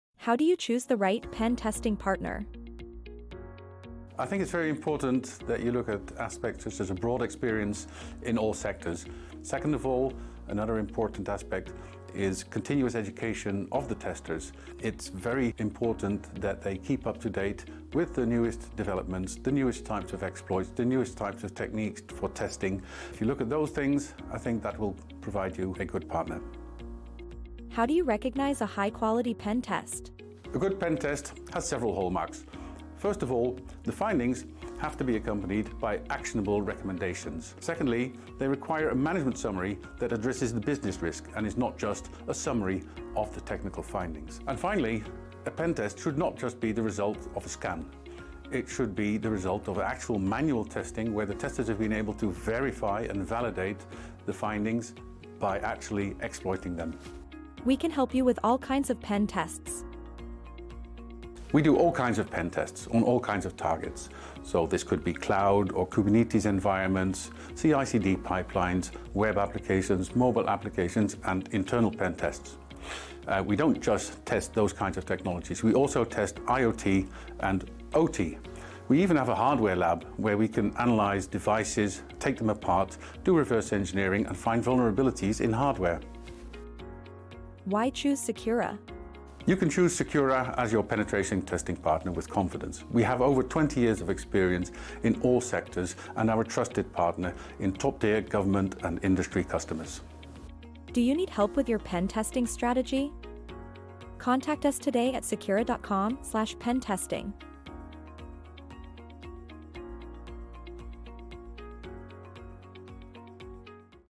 How to choose the right pentesting partner?
Vulnerability assessments and penetration testing (VA/PT) are ways to discover weak spots in the security of your website, mobile application, web application or infrastructure. It's not that hard to find a pentesting company. But how do you know a pentesting company delivers quality?
Secura's Technical Director Ralph Moonen explains how you can recognize a good pentest and what to look out for when choosing a pentesting partner.